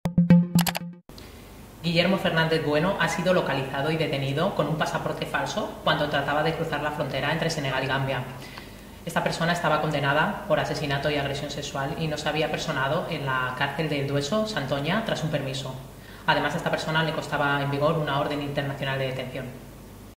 Detenido en Senegal el violador fugado de El Dueso
Vídeo: EFE